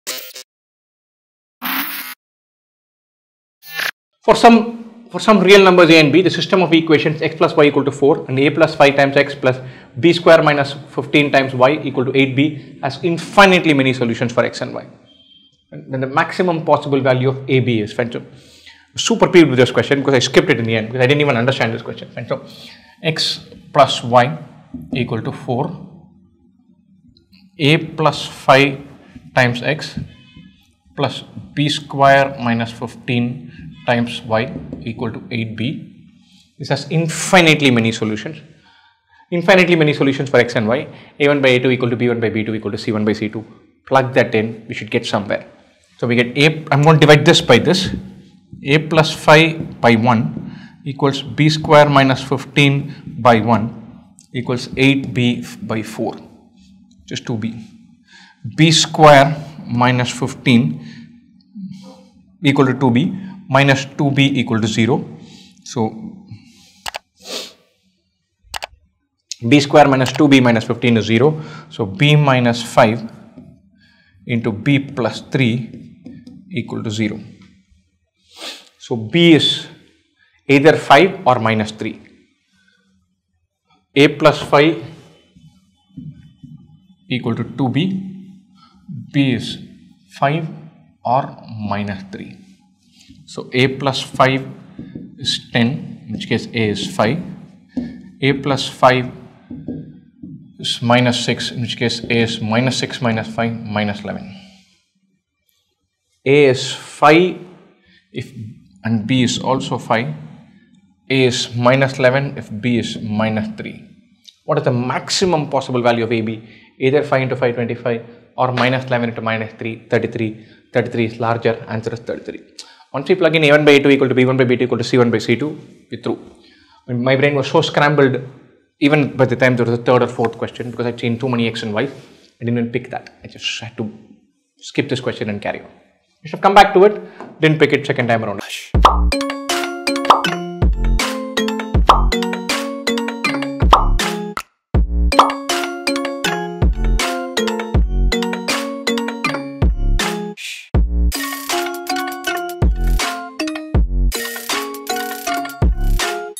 CAT 2023 Slot 3 | System of Equation | 2IIM CAT Prep | CAT 2024
For some real numbers a and b , the system of equations
x + y = 4 and 〖(𝒂+𝟓)𝒙+(𝒃〗^𝟐−𝟏𝟓)𝒚=𝟖𝒃 has infinitely many solutions for x and y . Then, the maximum possible value of ab is:
A) 15
B) 55
C) 33
D) 25